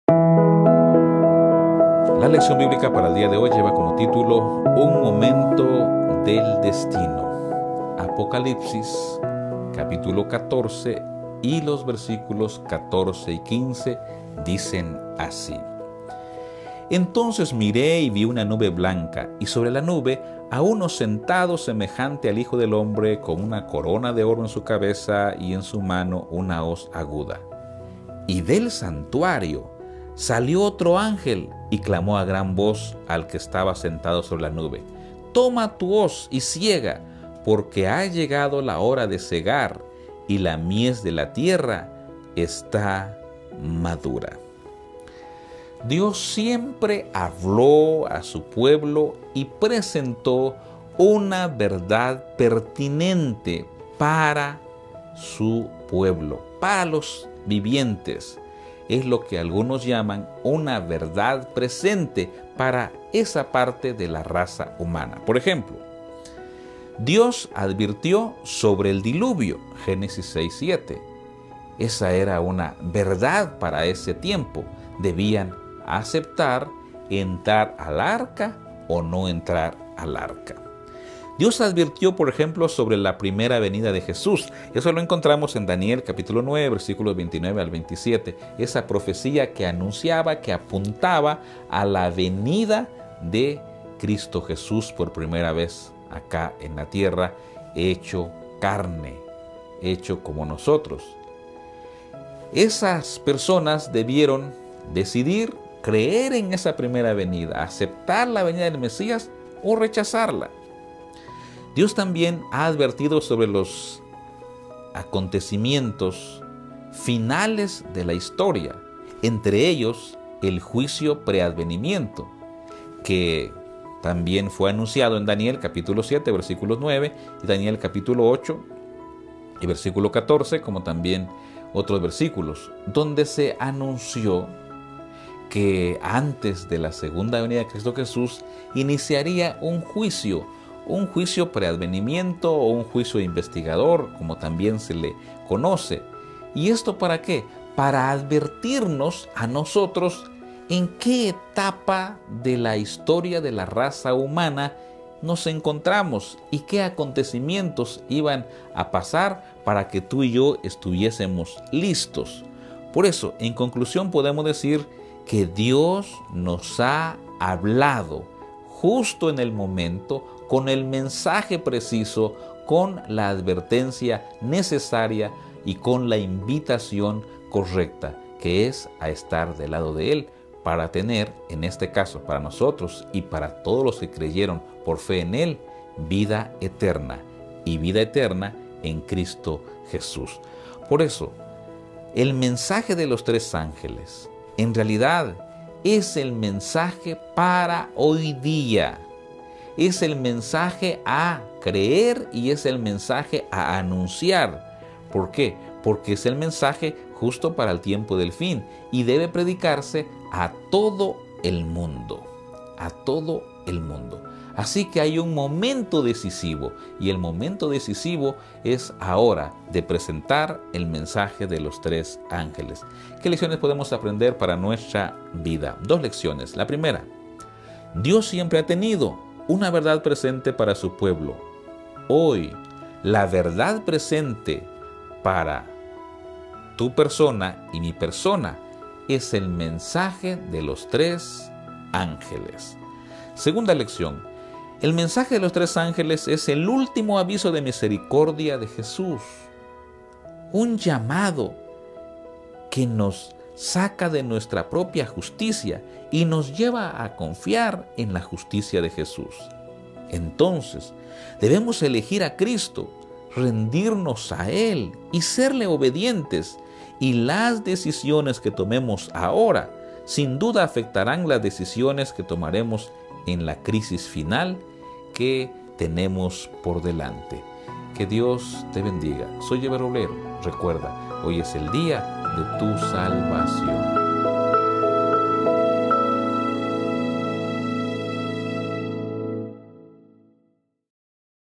LDB - UN MOMENTO DEL DESTINO - 7 DE ABRIL 2023
La lección para este día se titula: UN MOMENTO DEL DESTINO, un tema presentado por el Pastor Ever Roblero.